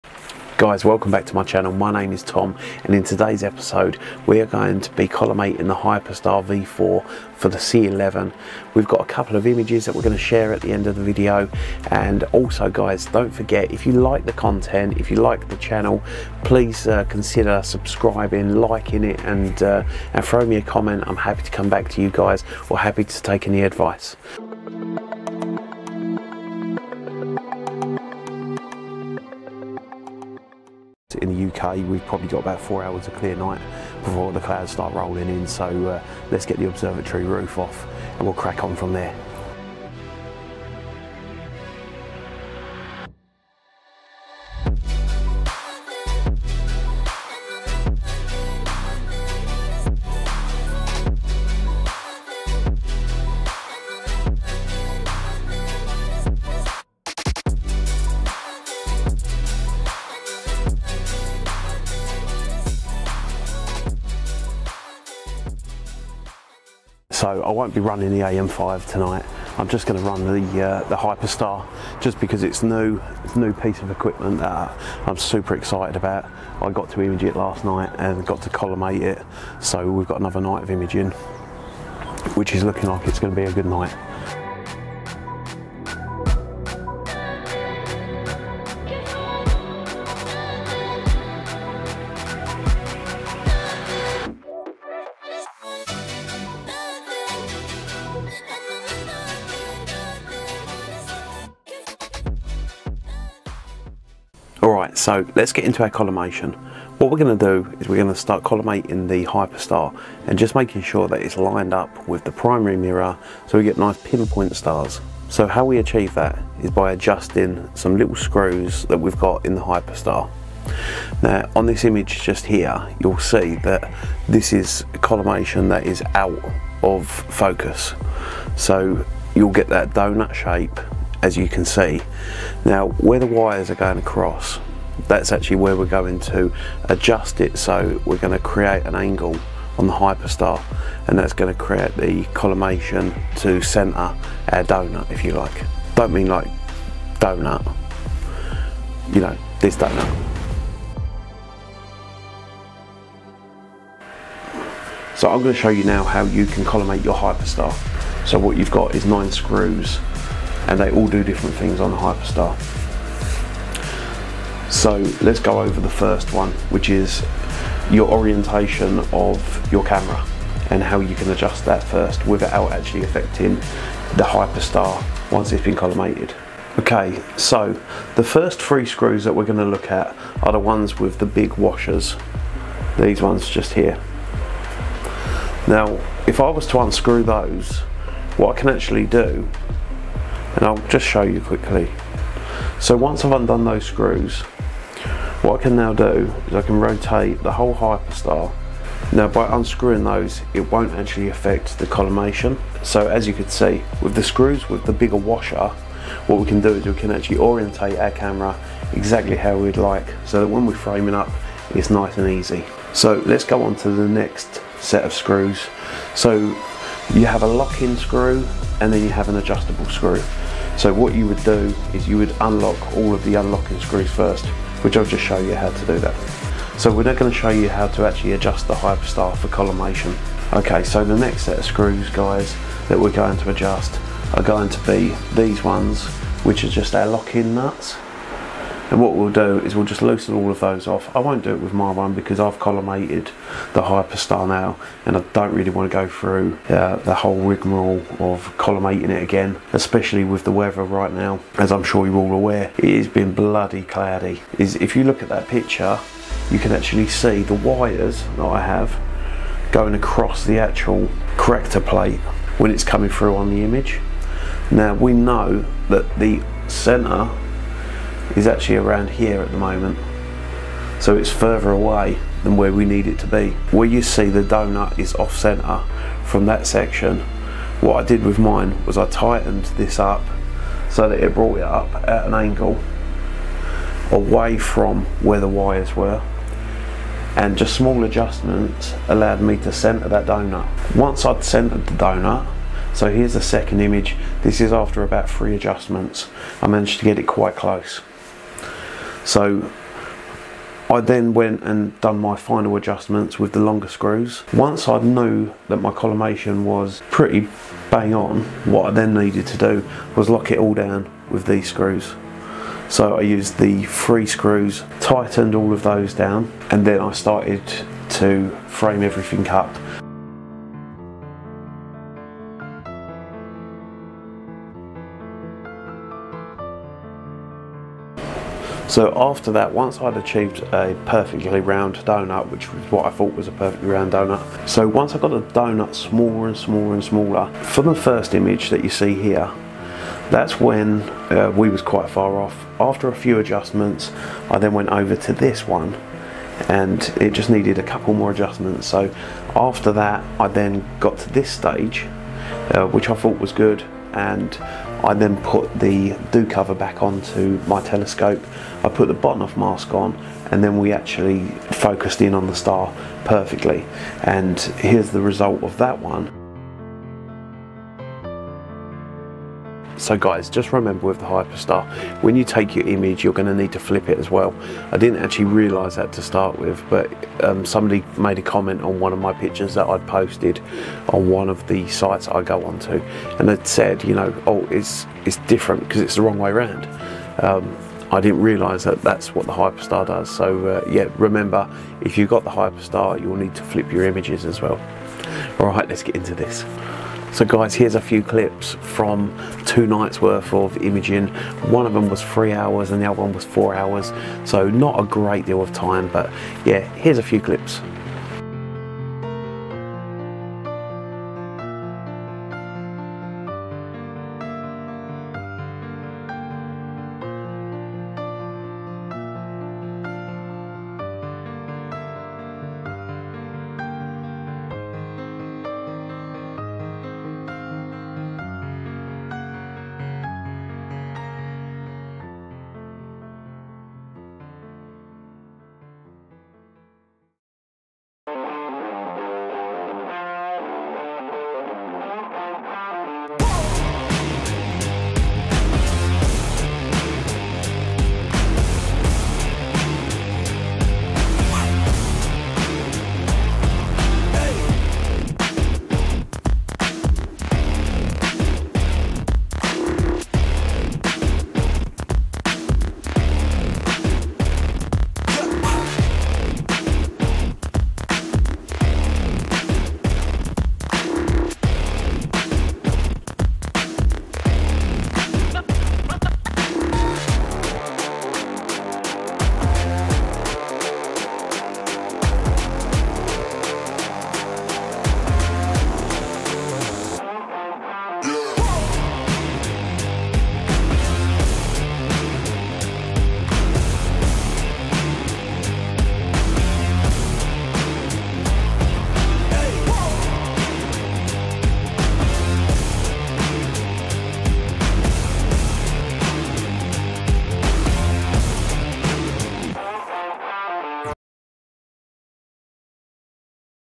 How to collimate the Hyperstar
in today's video we collimate the hyperstar and show first light. our image of Orions nebula and M81 and M82 using 30 second exposures show the light gathering of this monster.